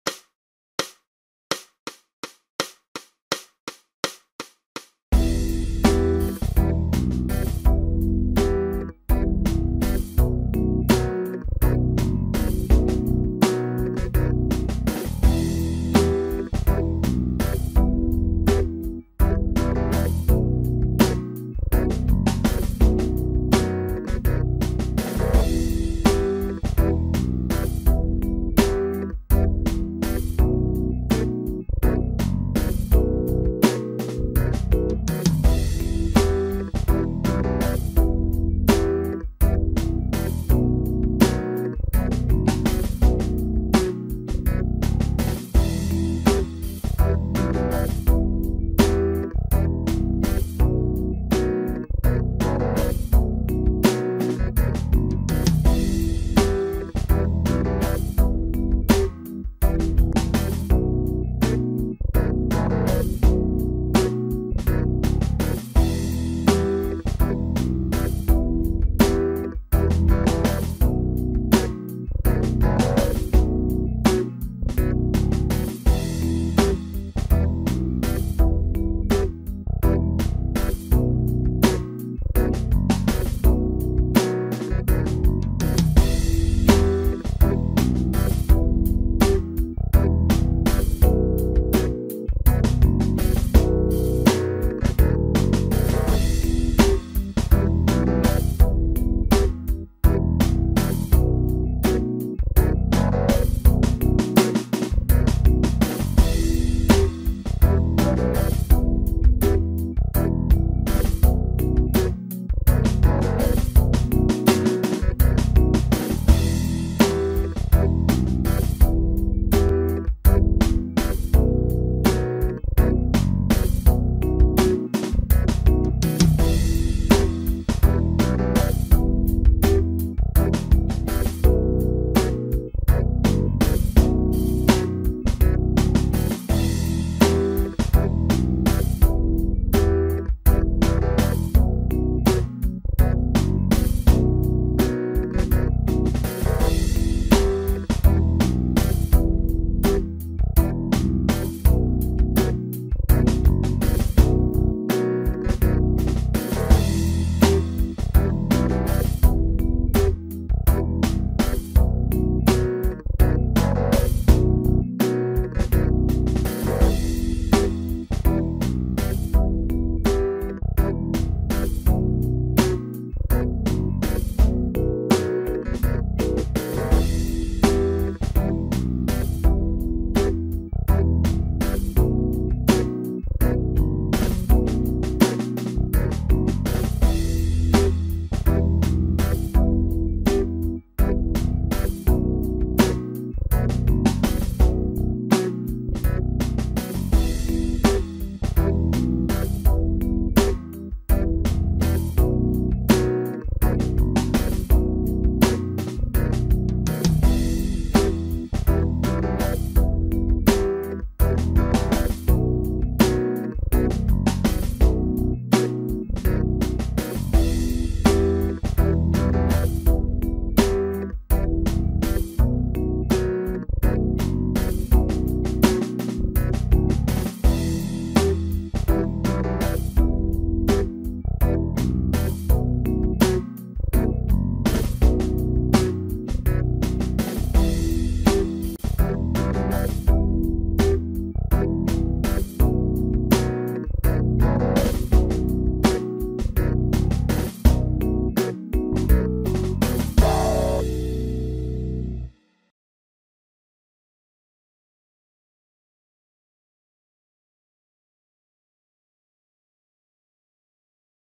Em Minor Rock in 78 Rock Backing Track Jam 164 BPM
Recommended Scales to use over this Track
E Harmonic Minor